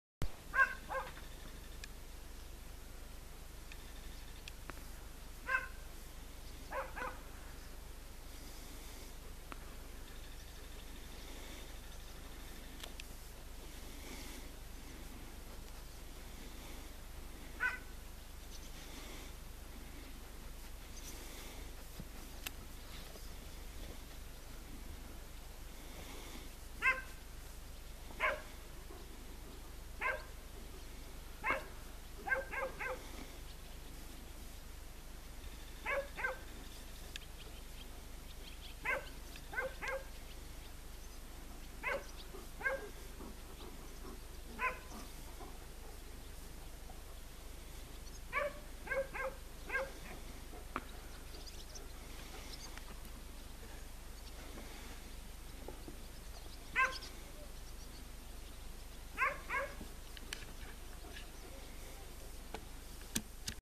Jeleń europejski
Nieśmiało rozpoczyna się rykowisko. Nocami słychać porykiwania byków. Do prawdziwych godów jeszcze chwila.